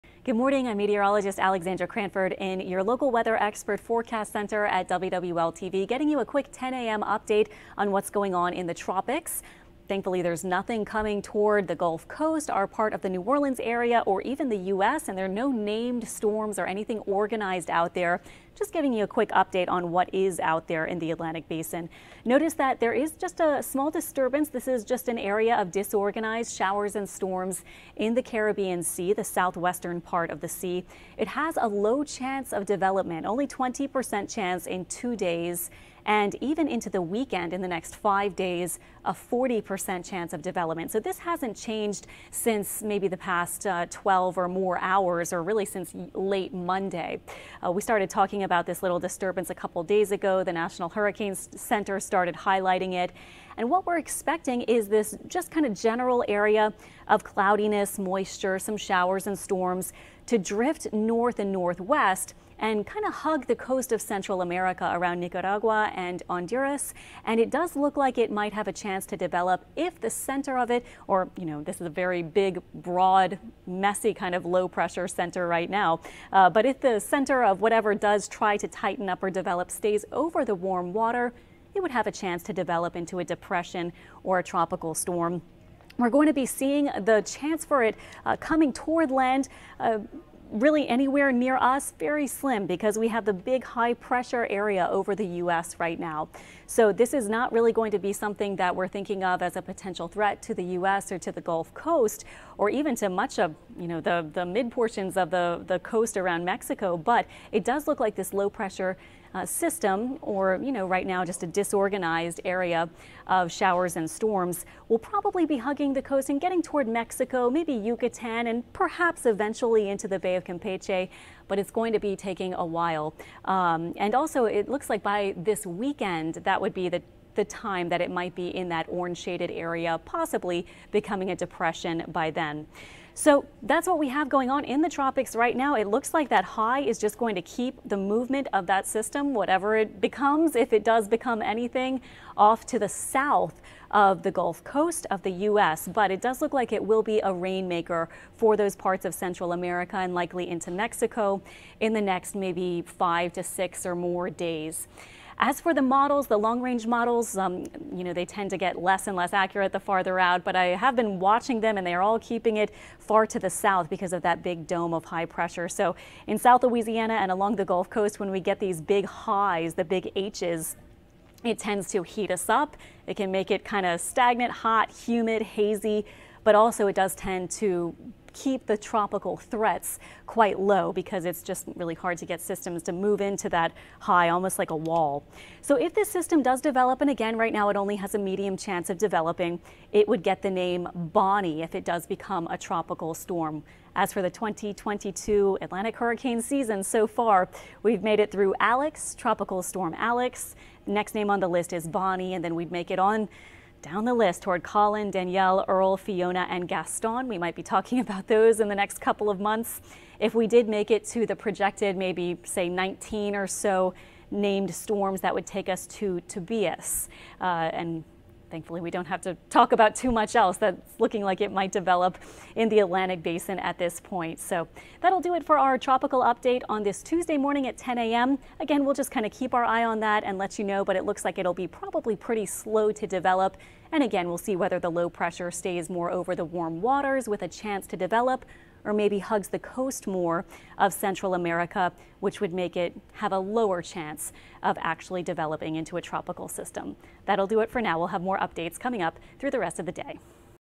Tuesday Morning Tropics Update: Watching development in Caribbean
A trough of low pressure in the southwest Caribbean Sea has a medium chance of development late this week. It could become a depression by this weekend as it drifts northwest along the coast of Central America and eventually into Mexico. The big high pressure dome over us will block any tropical systems from getting near the US Gulf Coast.